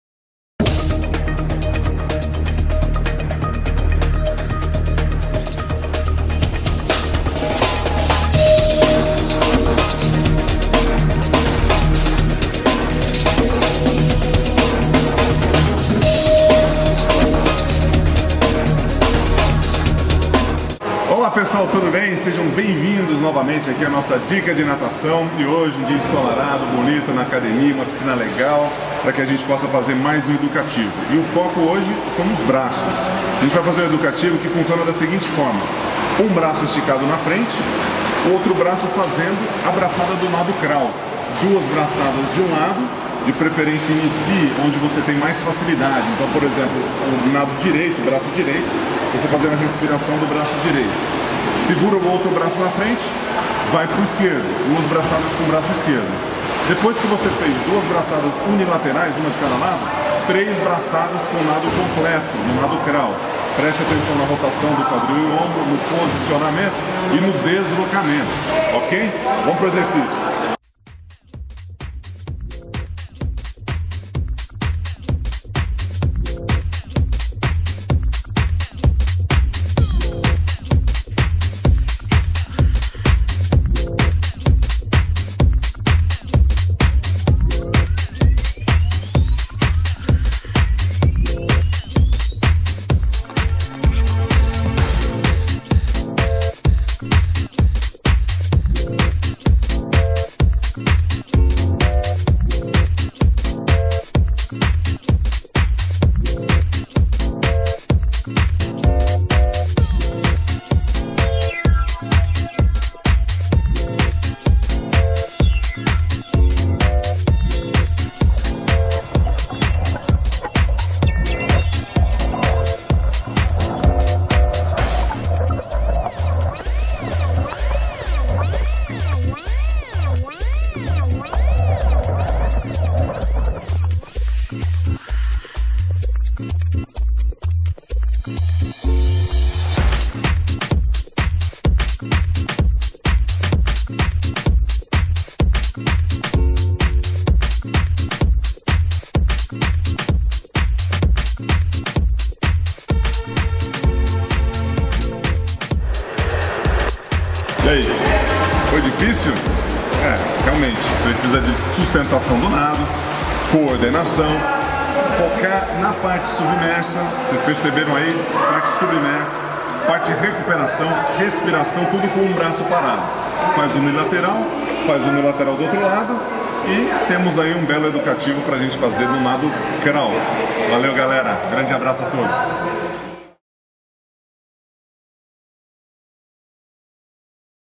[DICAS] Dica de NADO CRAWL - Dicas de Natação com Gustavo Borges
Trabalho alternado de técnica das braçadas. Lado esquerdo e direito.